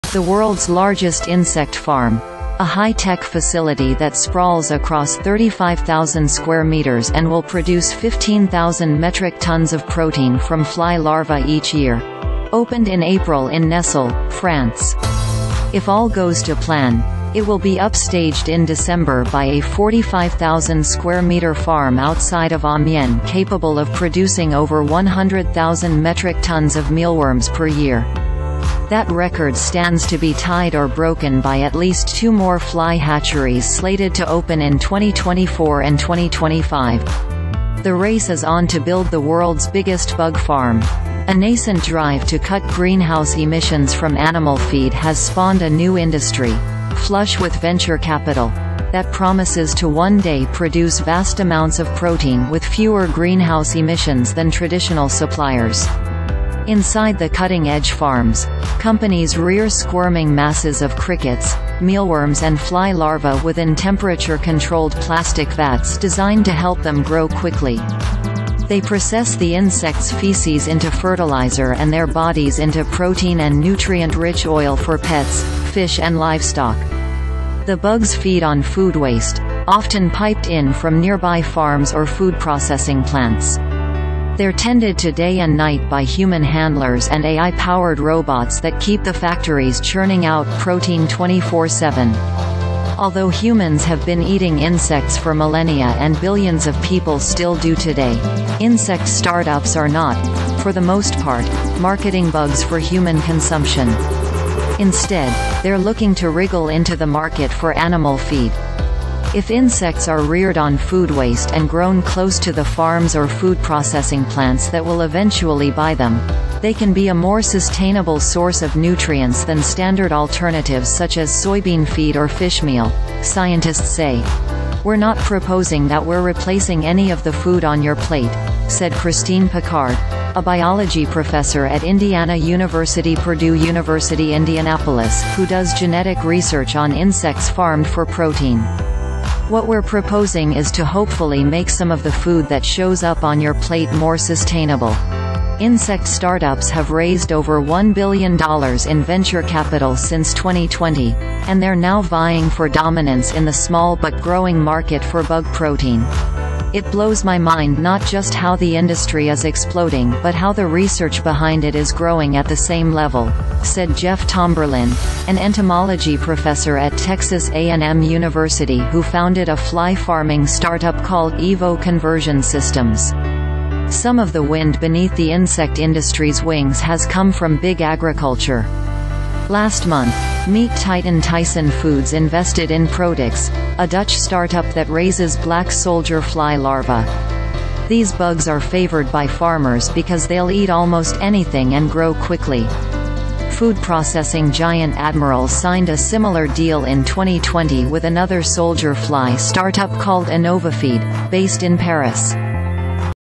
Why companies are racing to build the world's biggest bug farm
The world’s largest insect farm — a high-tech facility that sprawls across 35,000 square meters and will produce 15,000 metric tons of protein from fly larvae each year — opened in April in Nesle, France. If all goes to plan, it will be upstaged in December by a 45,000-square-meter farm outside of Amiens capable of producing over 100,000 metric tons of mealworms per year. That record stands to be tied or broken by at least two more fly hatcheries slated to open in 2024 and 2025. The race is on to build the world’s biggest bug farm. A nascent drive to cut greenhouse emissions from animal feed has spawned a new industry, flush with venture capital, that promises to one day produce vast amounts of protein with fewer greenhouse emissions than traditional suppliers. Inside the cutting-edge farms, companies rear squirming masses of crickets, mealworms and fly larvae within temperature-controlled plastic vats designed to help them grow quickly. They process the insects’ feces into fertilizer and their bodies into protein and nutrient-rich oil for pets, fish and livestock. The bugs feed on food waste, often piped in from nearby farms or food processing plants. They’re tended to day and night by human handlers and AI-powered robots that keep the factories churning out protein 24/7. Although humans have been eating insects for millennia and billions of people still do today, insect start-ups are not, for the most part, marketing bugs for human consumption. Instead, they’re looking to wriggle into the market for animal feed. If insects are reared on food waste and grown close to the farms or food processing plants that will eventually buy them, they can be a more sustainable source of nutrients than standard alternatives such as soybean feed or fishmeal, scientists say.“We’re not proposing that we’re replacing any of the food on your plate,” said Christine Picard, a biology professor at Indiana University-Purdue University Indianapolis who does genetic research on insects farmed for protein. “What we’re proposing is to hopefully make some of the food that shows up on your plate more sustainable.”Insect start-ups have raised over $1 billion in venture capital since 2020, and they’re now vying for dominance in the small but growing market for bug protein.“It blows my mind not just how the industry is exploding but how the research behind it is growing at the same level,” said Jeff Tomberlin, an entomology professor at Texas A&M University who founded a fly farming start-up called EVO Conversion Systems. Some of the wind beneath the insect industry’s wings has come from Big Agriculture. Last month, meat titan Tyson Foods invested in Protix, a Dutch start-up that raises black soldier fly larvae. These bugs are favored by farmers because they’ll eat almost anything and grow quickly. Food processing giant ADM signed a similar deal in 2020 with another soldier fly start-up called Innovafeed, based in Paris.